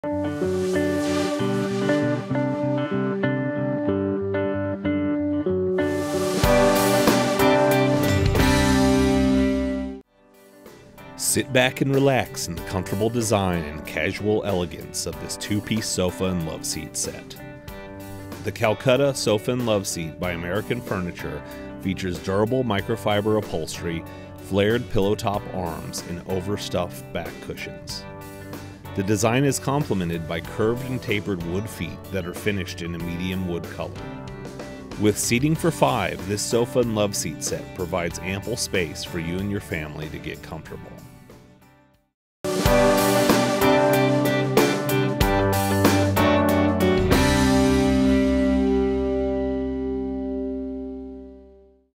American Furniture Camel Calcutta Sofa and Loveseat Set
Sit back and relax in the comfortable design and casual elegance of this two piece sofa and loveseat set.

The Calcutta Sofa and Loveseat by American Furniture Manufacturing, features durable microfiber upholstery, flared pillow-top arms and overstuffed back cushions.

The design is complimented by curved and tapered wood feet that are finished in a medium wood color.

With seating for five, this sofa and loveseat set provides ample space for you and your family to get comfortable.